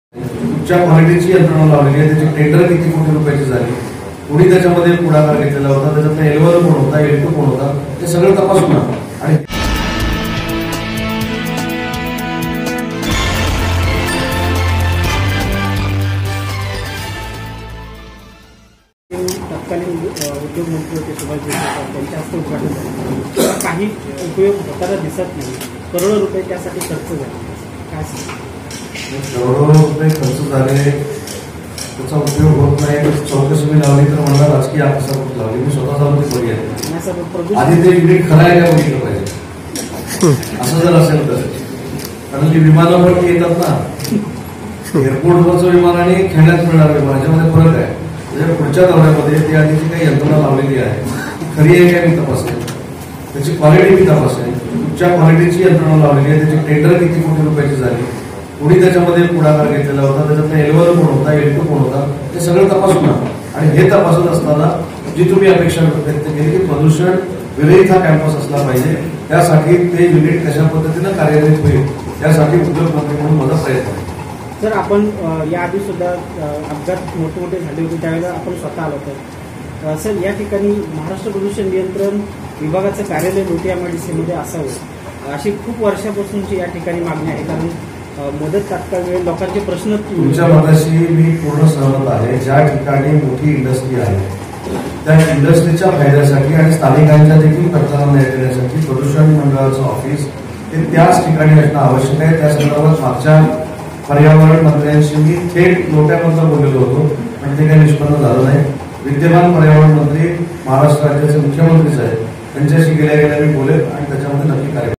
लोटेतील एअर मॉनिटरिंग सिस्टीमची चौकशी करणार..
उद्योगमंत्री ना. उदय सामंत